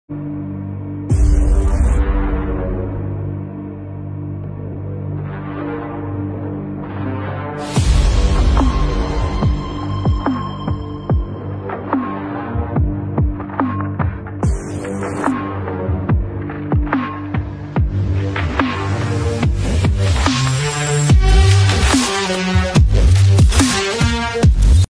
Belgorod - 20.10.2024. Georgian Recon Unit - Mimino Burned A RuAF T-80 Tank In The Belgorod Oblast.
📌Georgian Recon Unit (Part Of International Legion) - Mimino Burned A RuAF T-80 Tank In The Belgorod Oblast.⤵️